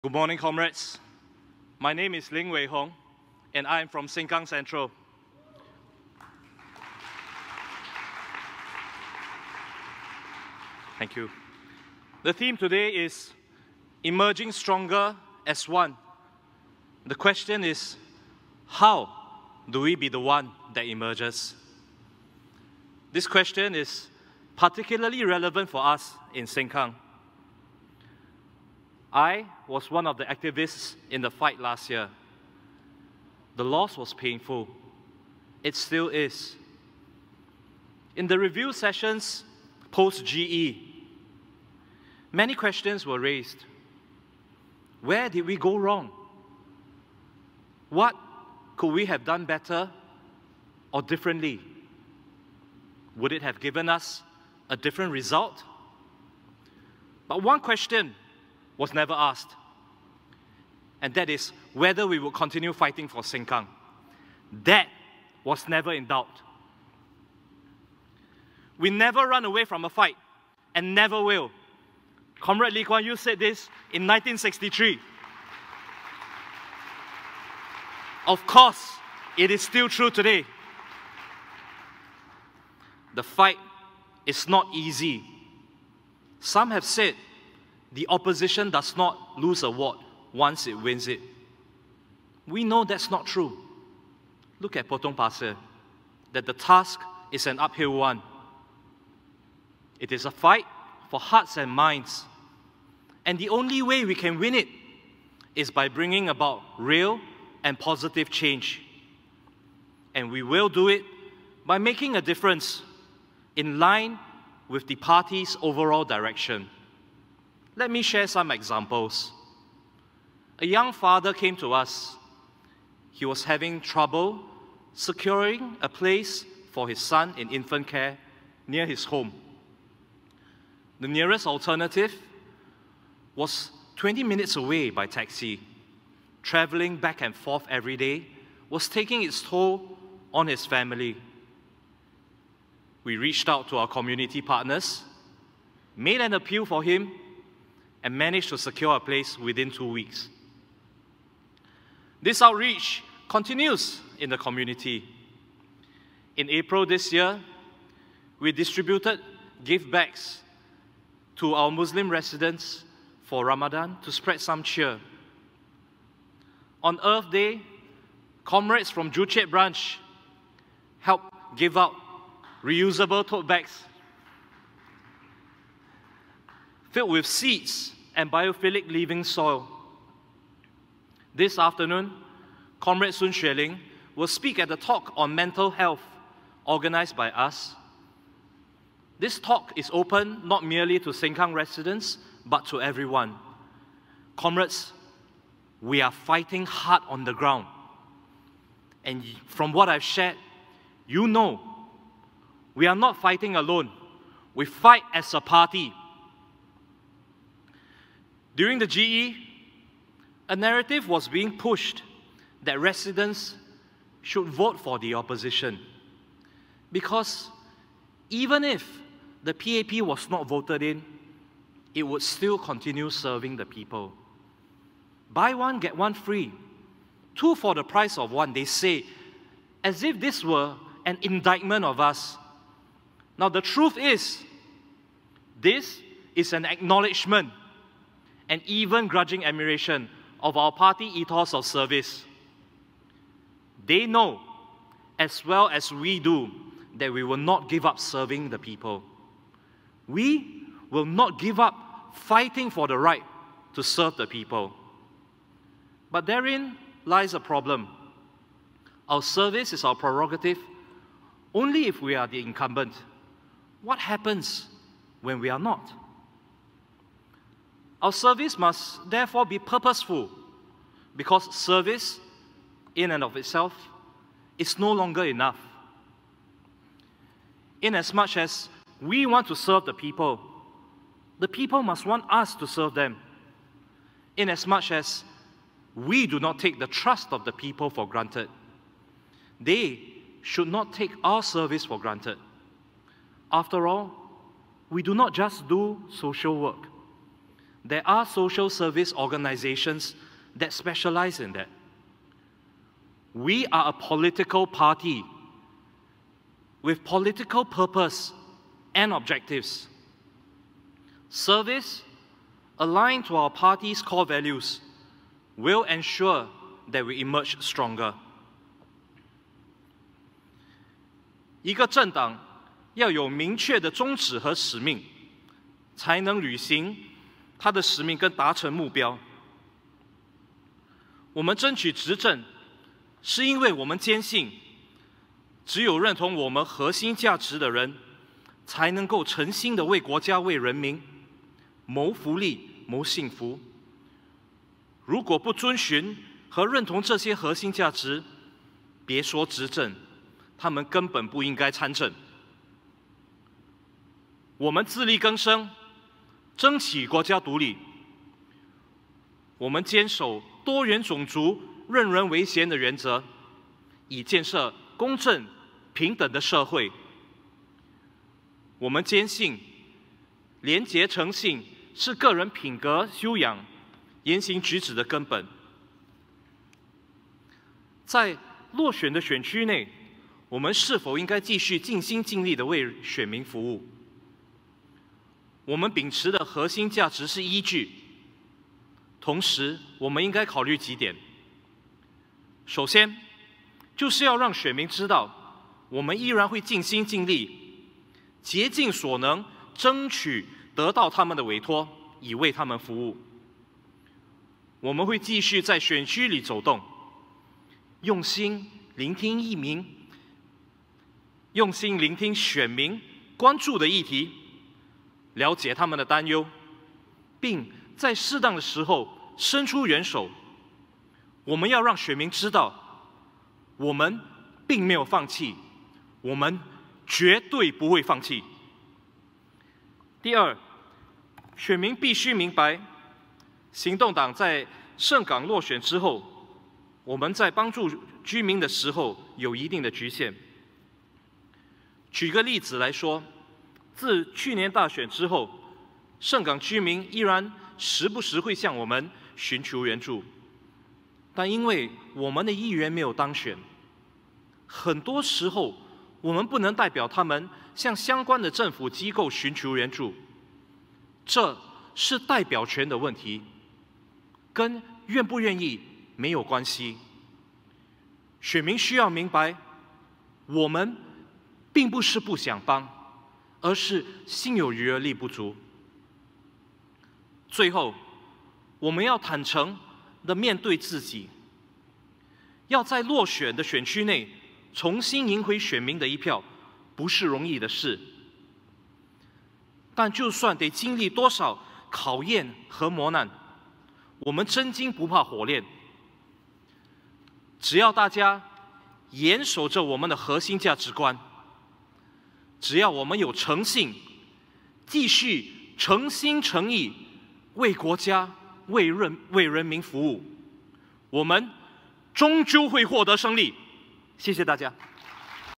Sengkang GRC activist Ling Wei Hong speaks at PAP convention
Sengkang GRC activist Ling Wei Hong, who spoke at the event, described the sense of loss felt by party activists when the constituency was won by the Workers' Party in last year's general election.

This year's PAP convention was held at Suntec Singapore Convention and Exhibition Centre on Nov 27-28, 2021.

VIDEO: PAP